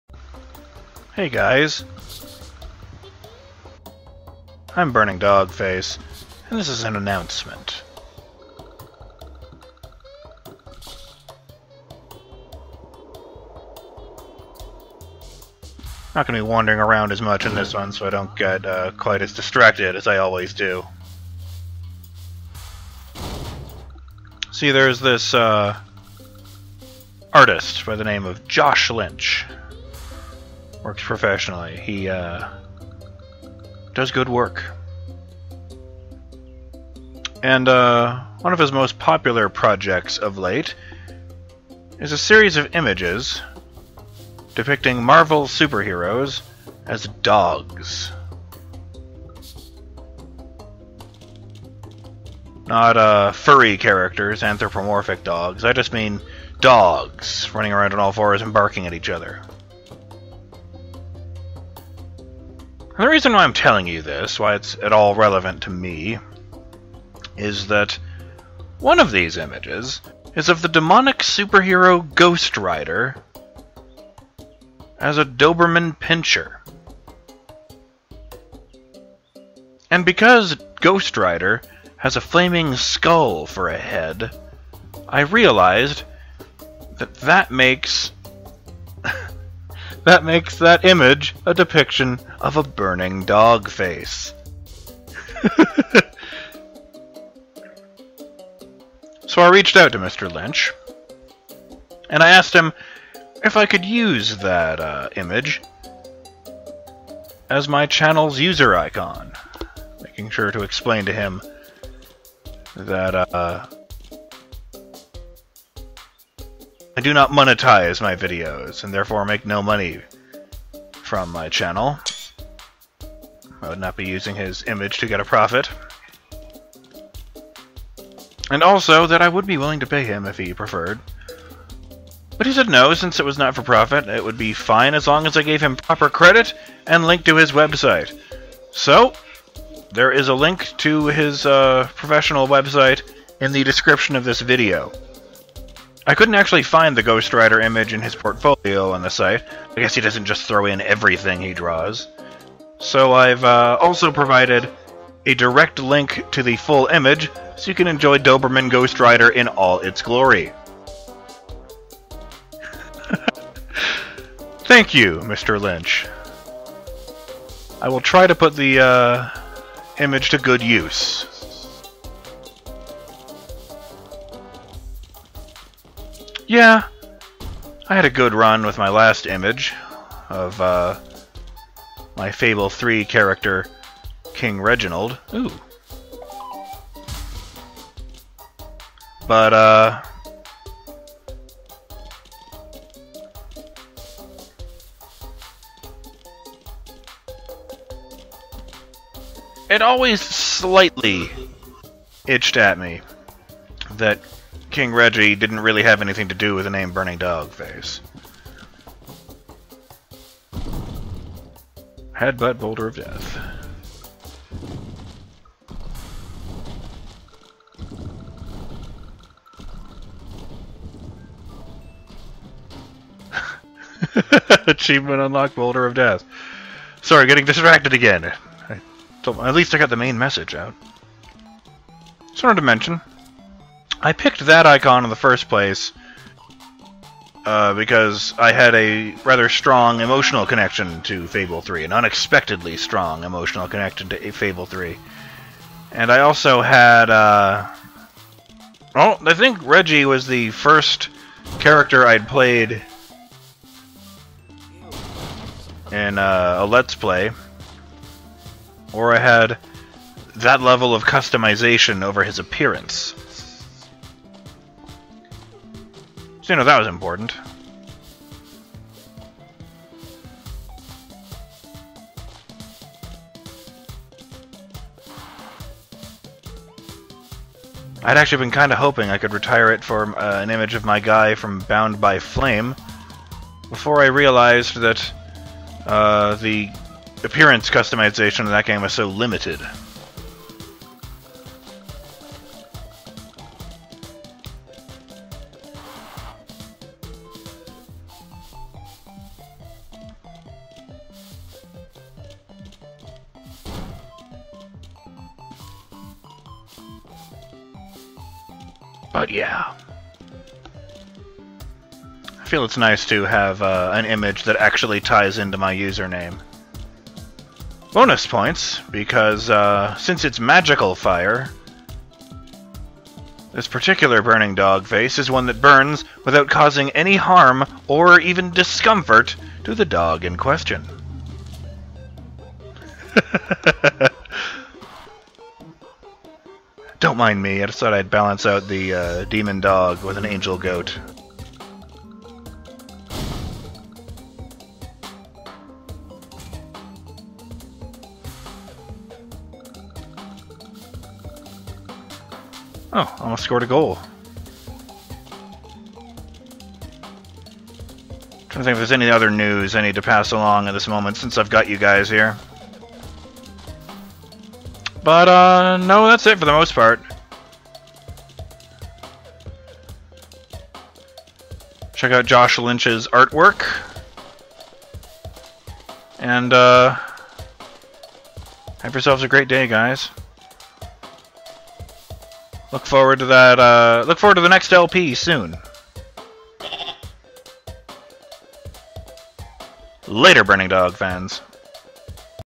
Announcement - A Change of Face
The time has come to retire King Reggie from Let's Play Fable III as my avatar. At long last, I really am BurningDogFace.